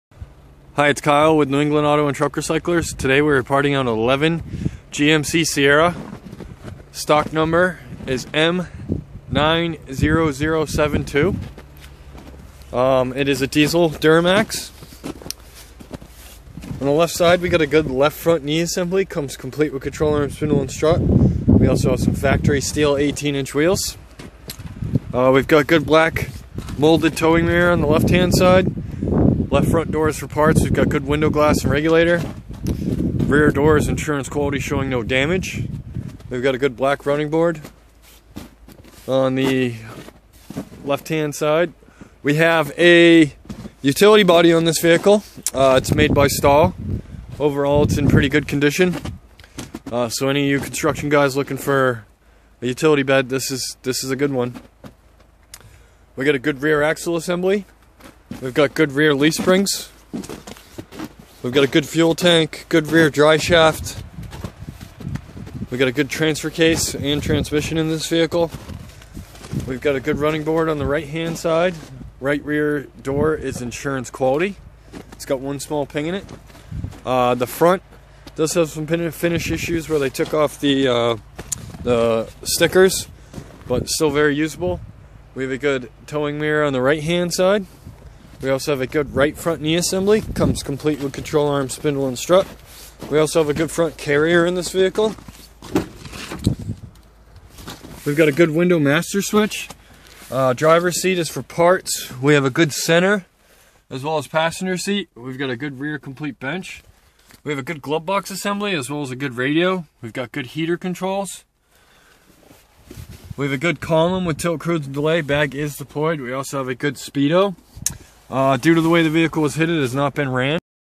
Parting out 2011 GMC Sierra 2500 Pickup | M90072
New England Auto & Truck Recyclers is parting out 2011 GMC Sierra 2500 Pickup